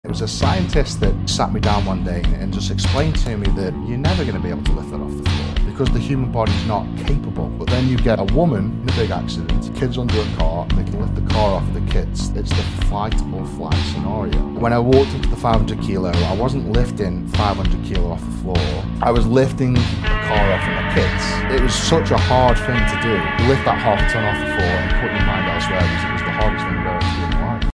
Surprising Story Behind the 500KG Deadlift! | Eddie Hall
Discover The Worlds Strongest Man's Secret to Crushing the 500 KG World Record Deadlift in this Inspirational YouTube Short!